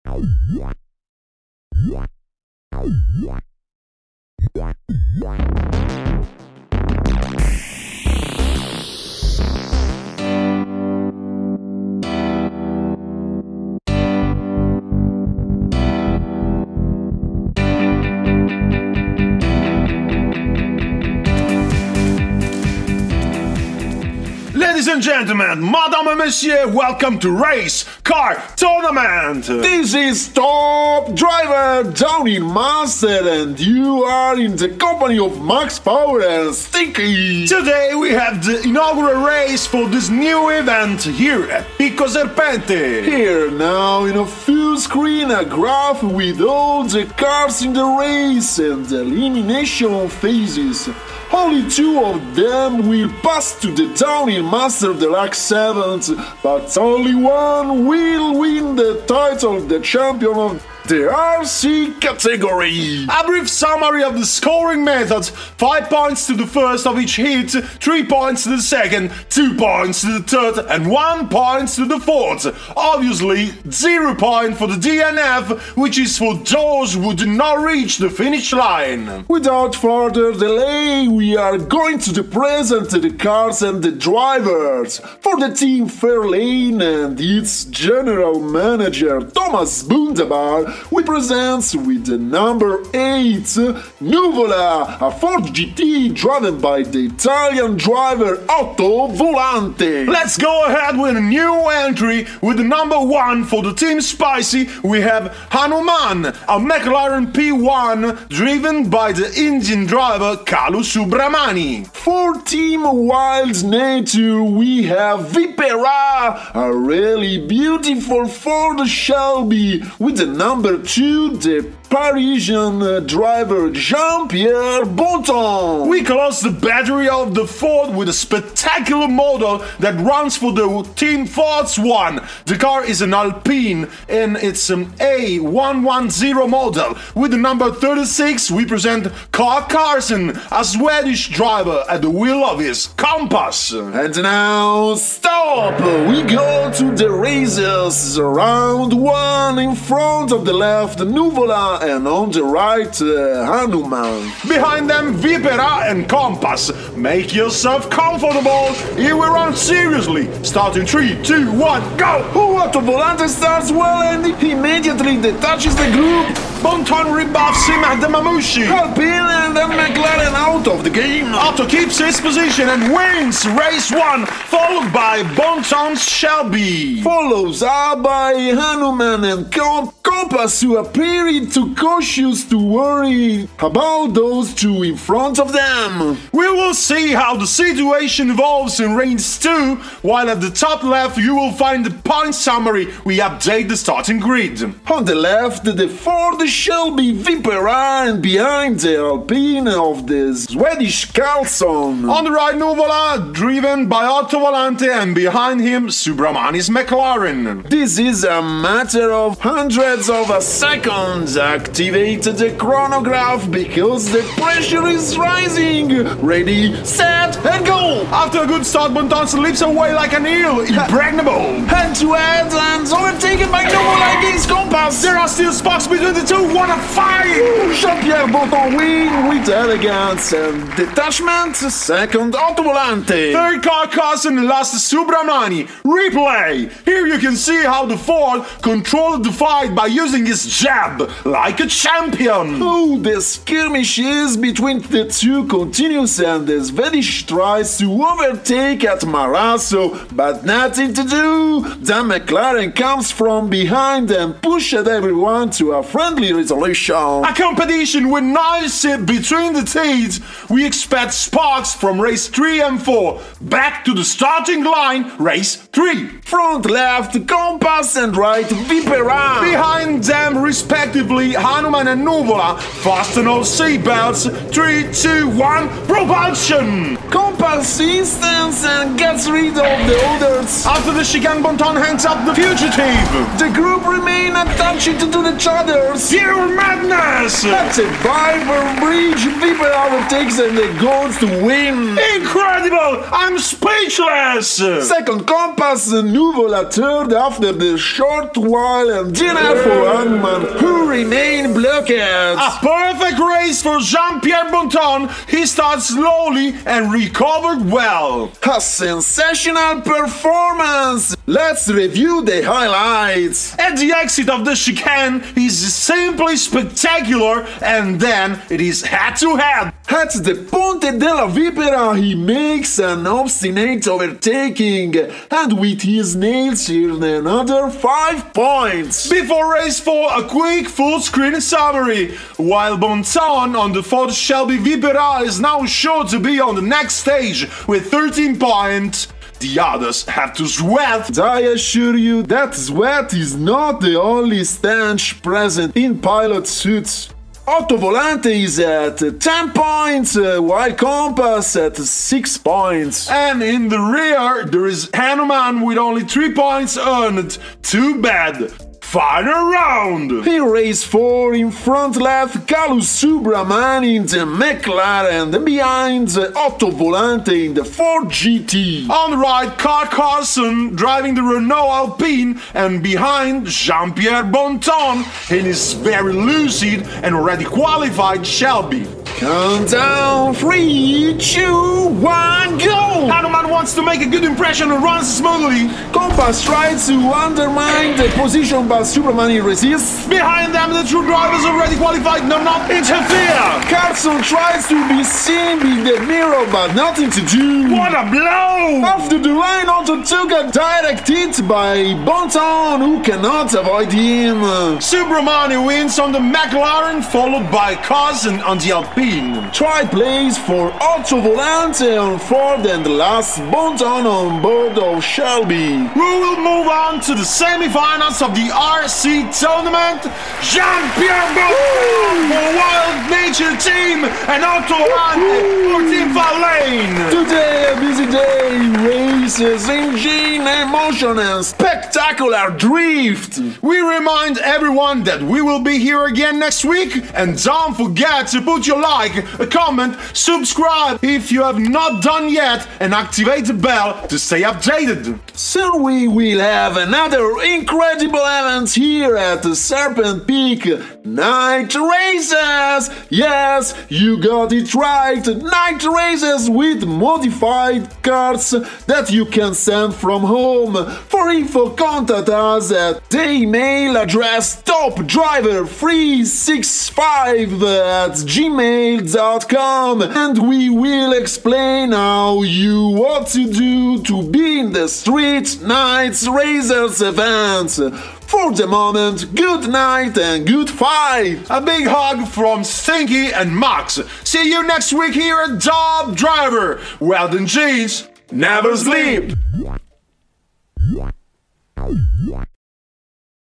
RC Tournament: EP.1 The butterfly effect. Top Driver Diecast Racing 1:64
Top Driver English version!!! Only for your eyes, Modified diecast cars 1:64 race track

0:00:24 Start!
0:01:24 Presentation
0:02:30 Round 1
0:03:15 Round 2
0:04:26 Round 3
0:05:59 Final Round
0:07:00 Winner!
0:07:20 Greetings!